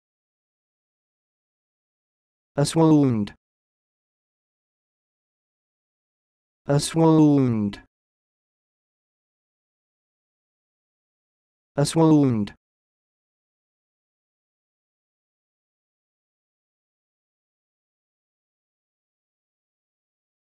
Learn how to say this word: "Aswooned"
This is the definition of "Aswooned":
adv.  In a swoon.